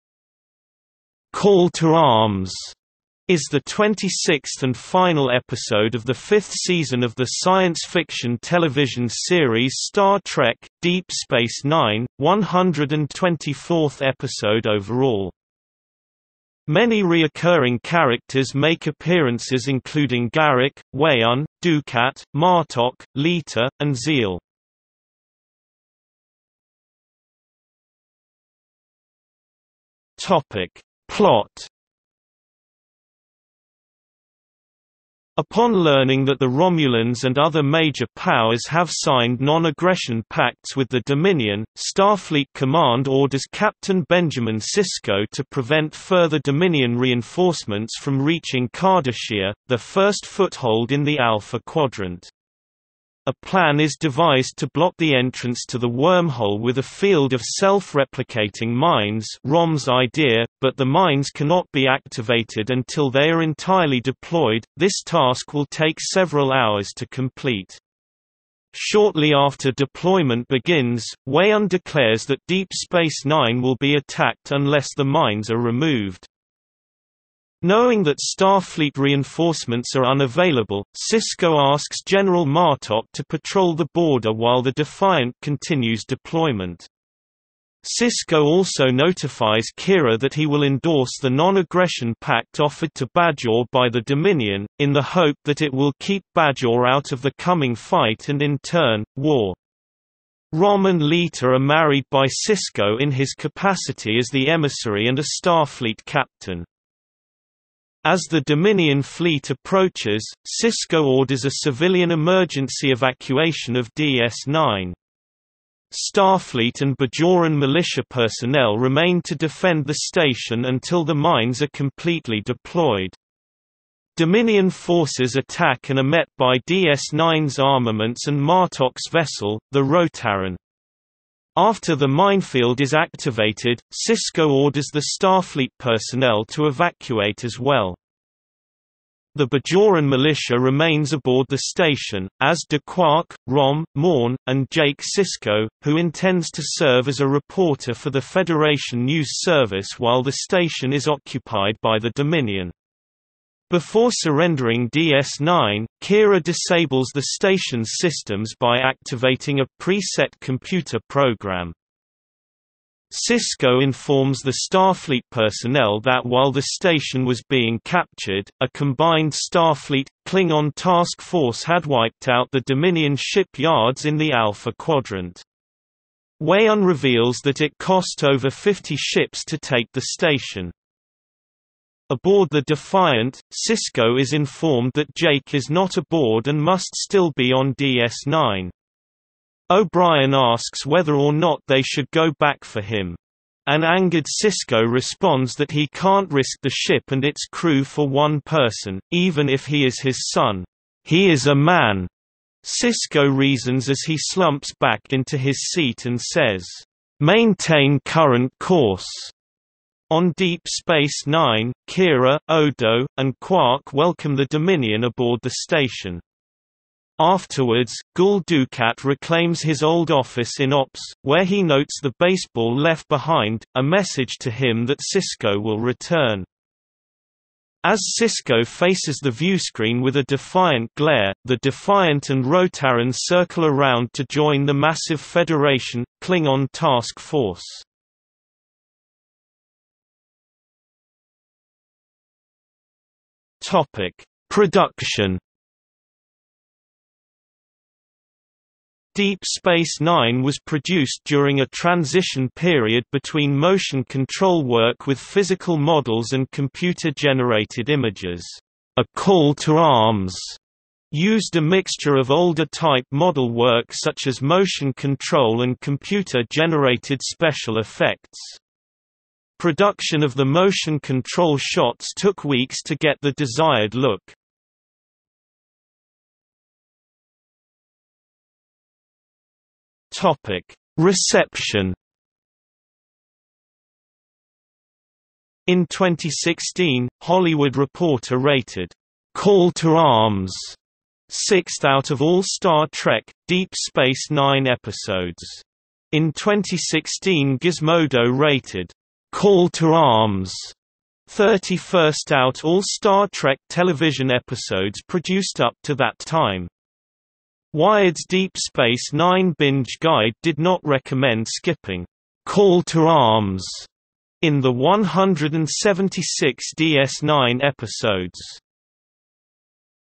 Call to Arms (Star Trek: Deep Space Nine) | Wikipedia audio article
00:00:26 1 Plot
00:04:00 2 Production
00:04:36 3 Reception

- Socrates

SUMMARY
"Call to Arms" is the 26th and final episode of the fifth season of the science fiction television series Star Trek: Deep Space Nine, 124th episode overall.
Many reoccurring characters make  appearances including Garak, Weyoun, Dukat, Martok, Leeta, and Ziyal.